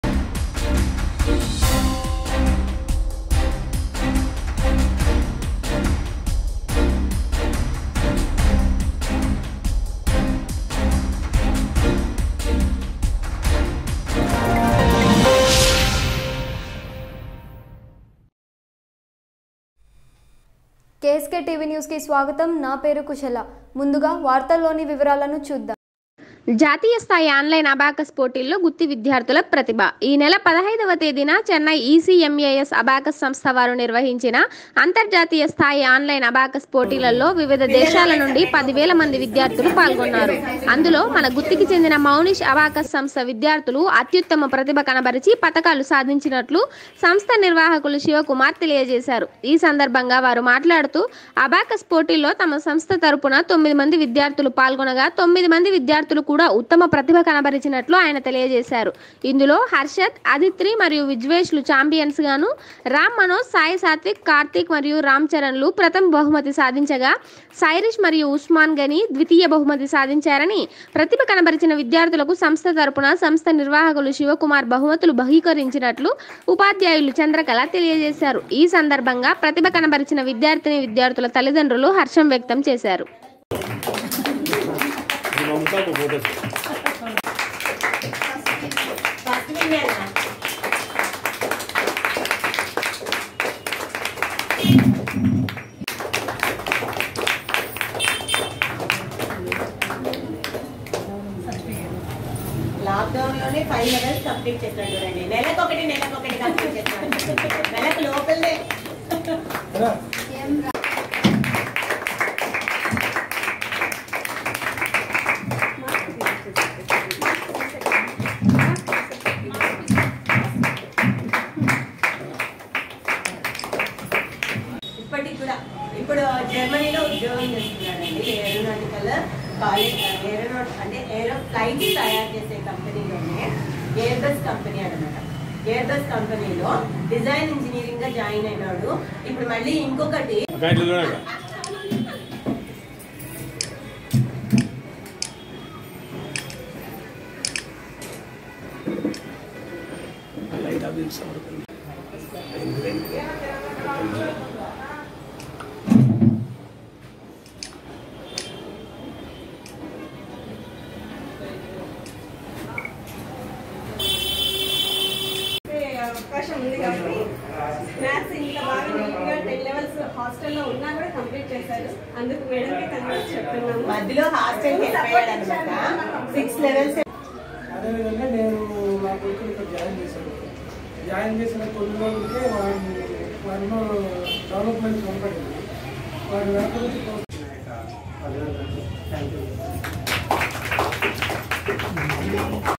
జాతీయ స్థాయి ఆన్ లైన్ అబాకస్ పోటీల్లో గుత్తి విద్యార్థుల ప్రతిభ.
ఈనెల 15వ తేదీన చెన్నై ఈసిఎంఏఎస్ అబాకస్ సంస్థ వారు నిర్వహించిన అంతర్జాతీయ స్థాయి ఆన్లైన్ అబాకస్ పోటీలలో వివిధ దేశాల నుండి పదివేల మంది విద్యార్థులు పాల్గొన్నారు.అందులో మన గుత్తికి చెందిన మౌనిష్ అబాకస్ సంస్థ విద్యార్థులు అత్యుత్తమ ప్రతిభ కనబరిచి పథకాలు సాధించినట్లు సంస్థ నిర్వాహకులు శివ కుమార్ తెలియజేశారు.ఈ సందర్భంగా వారు మాట్లాడుతూ అబాకస్ పోటీల్లో తమ సంస్థ తరఫున తొమ్మిది మంది విద్యార్థులు పాల్గొనగా తొమ్మిది మంది విద్యార్థులు కూడా ఉత్తమ ప్రతిభ కనబరిచినట్లు ఆయన తెలియజేశారు.ఇందులో హర్షద్, అధిత్రి మరియు విజ్వేష్ లు చాంపియన్స్ గాను రామమనోజ్, సాయి సాత్విక్, కార్తీక్ మరియు రామ్ చరణ్ లు ప్రథమ బహుమతి సాధించగా, సైరిష్ మరియు ఉస్మాన్ గని ద్వితీయ బహుమతి సాధించారని, ప్రతిభ కనబరిచిన విద్యార్థులకు సంస్థ తరుపున  సంస్థ నిర్వాహకులు శివకుమార్ బహుమతులు బహూకరించినట్లు ఉపాధ్యాయులు చంద్రకళ తెలియజేశారు.ఈ సందర్భంగా ప్రతిభ కనపరిచిన విద్యార్థినీ, విద్యార్థుల తల్లిదండ్రులు హర్షం వ్యక్తం చేశారు.